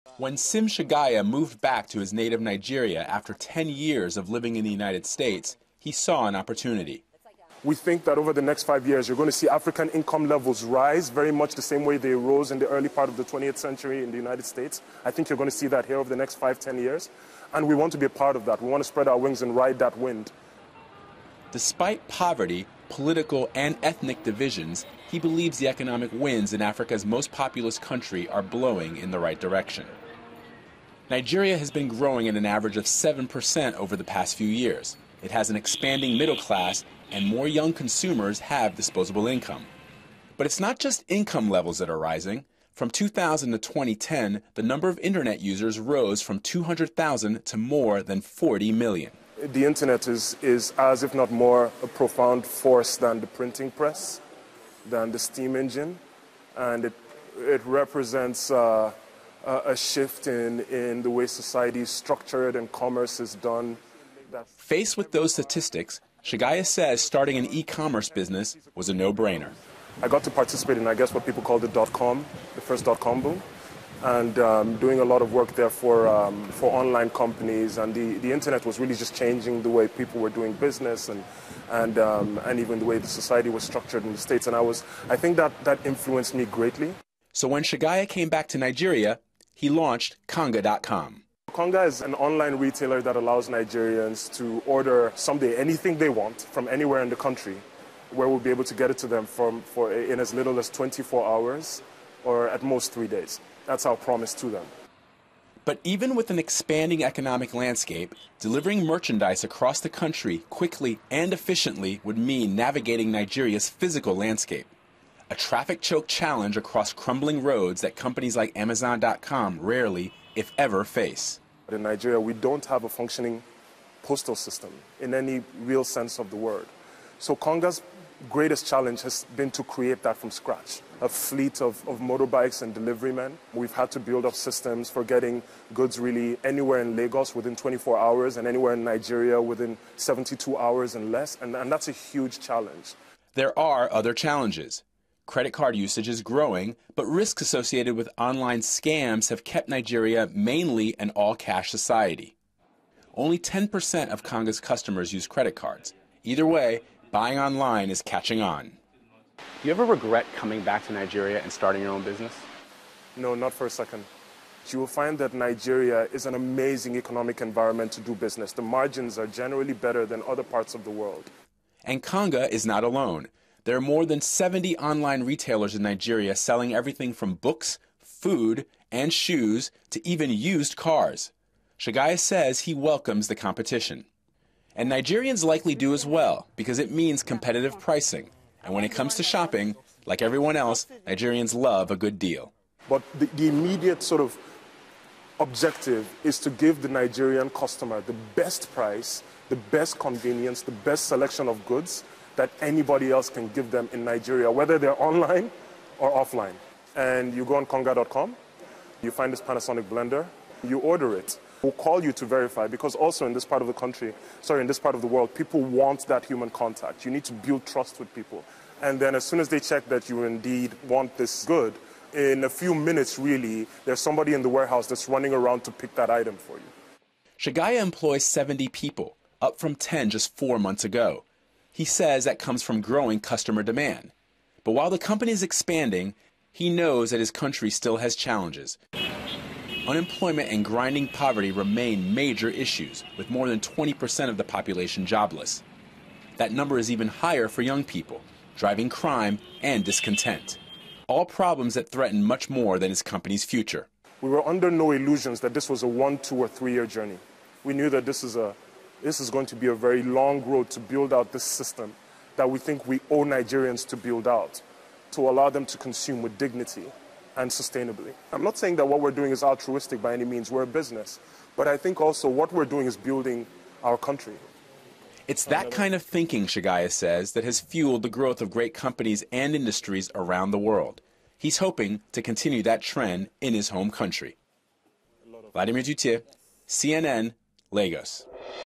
CNN Interview with Konga.com by Vladimir Duthiers
CNN's Vladimir Duthiers reports on how Nigeria's emerging middle class is turning to online shopping.